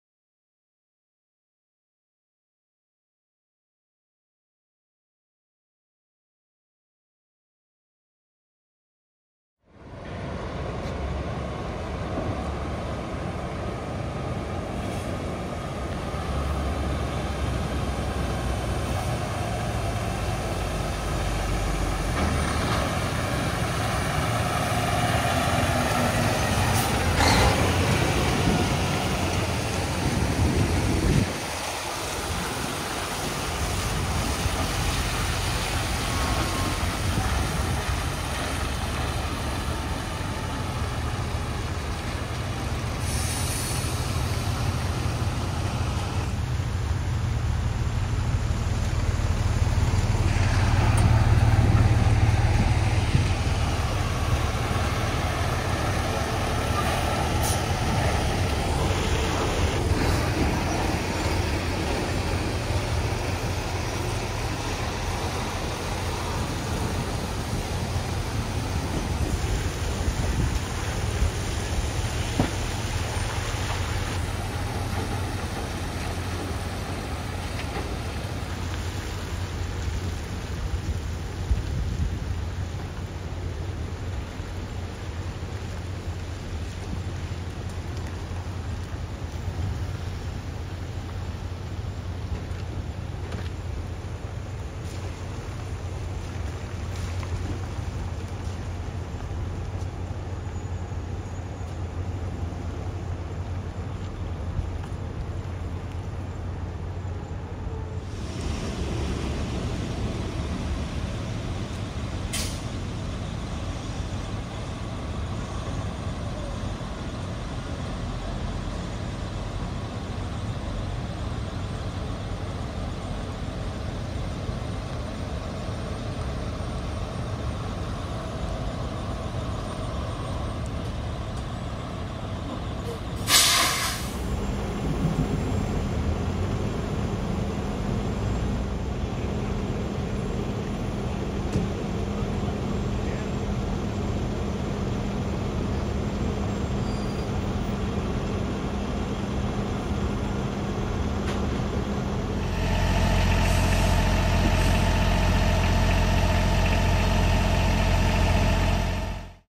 ⁴ᴷ ᴴᴰᴿ First Open Gangway R211T Subway Cars Arrive in New York City!
At long last, the highly anticipated "R211T" subway cars have made it to New York! The R211Ts are virtually identical to the already delivered R211A test train, but these cars have an "open gangway" design which will allow passengers to walk safely throughout the train cars without having to cross over a dangerous gap. The first two cars to be delivered were cars 4044 and 4043. In total, 20 open gangway cars have been ordered as a test to see if they would work within the system.